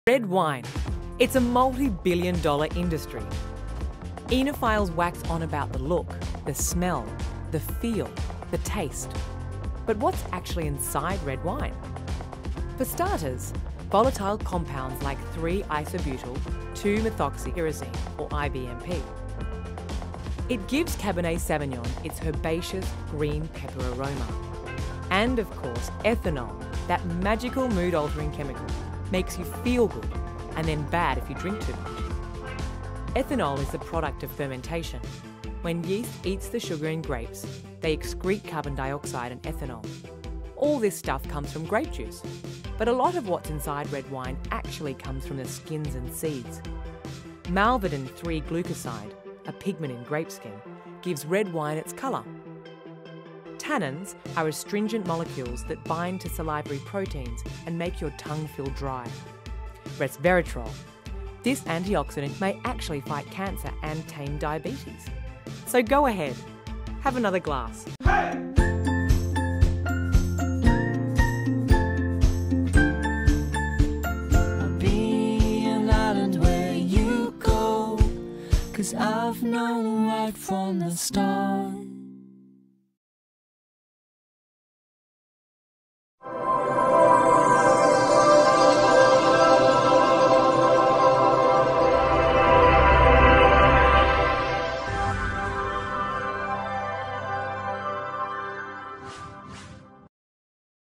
RED WINE Business Tip of the day. JT Ippolito
Red Wine
Oenophiles wax poetic about the look, feel, smell, and taste of red wine. But what’s actually inside the drink? Dozens of complicated molecules from the grape’s juice, seeds, and skin. Oh, and alcohol.